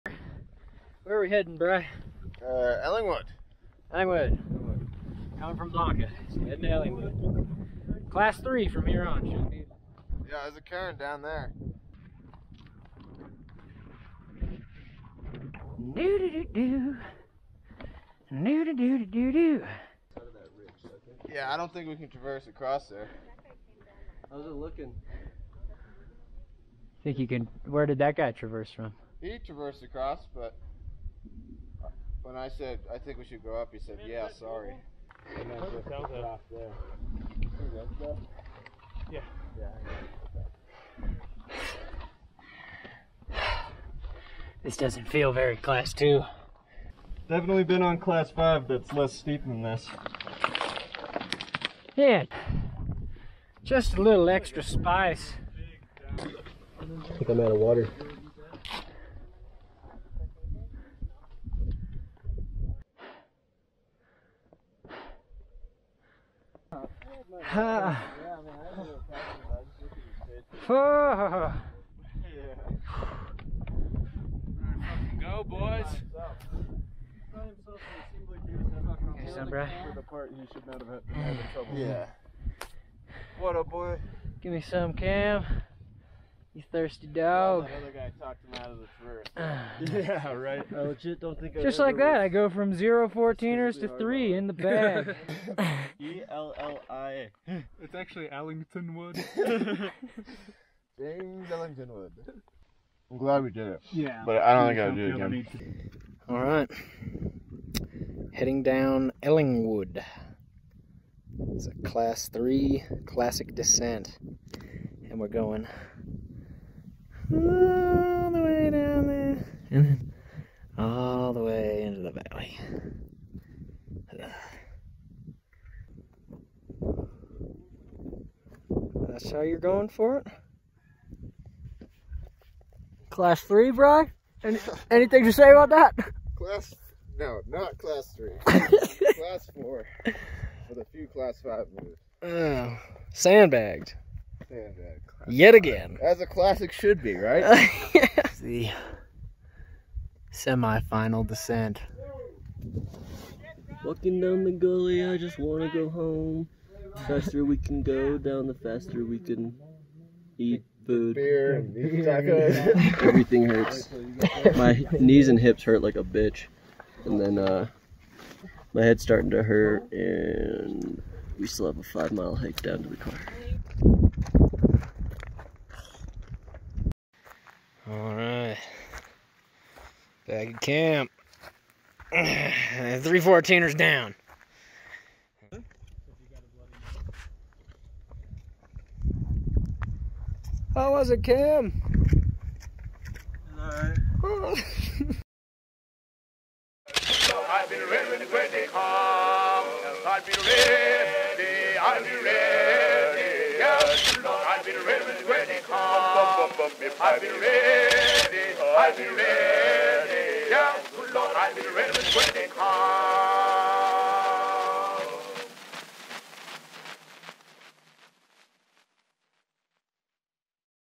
Colorado's Most Dangerous Traverse pt. 4 | EYT
Our heroes reach the final summit and survive the most technical bits of climbing. Stamina becomes a greater threat than exposure as they still have a grueling final descent to go.